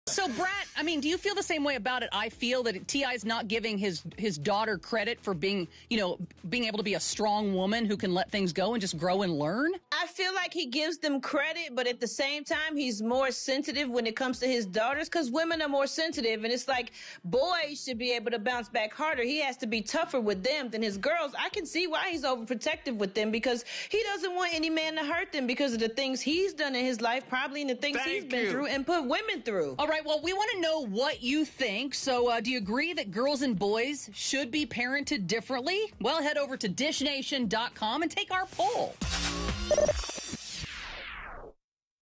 TAKE OUR POLL! DO YOU AGREE WITH T.I. THAT GIRLS & BOYS SHOULD BE PARENTED DIFFERENTLY?
T.I. sets the record straight on his parenting style, says he protects his daughters from heartbreak and NOT his sons 👀

Do you agree girls and boys should be parented differently? 🤔Head over to DishNation.com to take our poll!